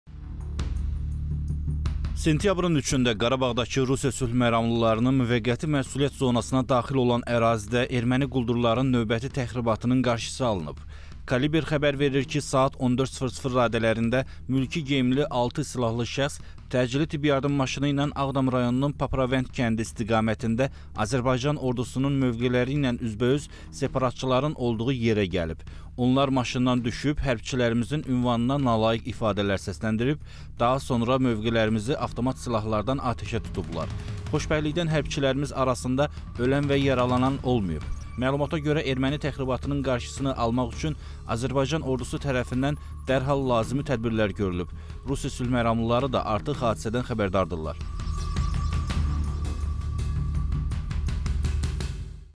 Mülki geyimli ermənilər hərbçilərimizə atəş açdı
Sentyabrın 3-də Qarabağdakı Rusiya sülhməramlılarının müvəqqəti məsuliyyət zonasına daxil olan ərazidə erməni quldurların növbəti təxribatının qarşısı alınıb.
“Caliber” xəbər verir ki, saat 14:00 radələrində mülki geyimli 6 silahlı şəxs təcili tibbi yardım maşını ilə Ağdam rayonunun Papravənd kəndi istiqamətində Azərbaycan Ordusunun mövqeləri ilə üzbəüz separatçıların olduğu yerə gəlib. Onlar maşından düşüb, hərbçilərimizin ünvanına nalayiq ifadələr səsləndirib, daha sonra mövqelərimizi avtomat silahlardan atəşə tutublar. Xoşbəxtlikdən hərbçilərimiz arasında ölən və yaralanan olmayıb.
Məlumata görə, erməni təxribatının qarşısını almaq üçün Azərbaycan Ordusu tərəfindən dərhal lazımi tədbirlər görülüb. Rusiya sülhməramlıları da artıq hadisədən xəbərdardırlar.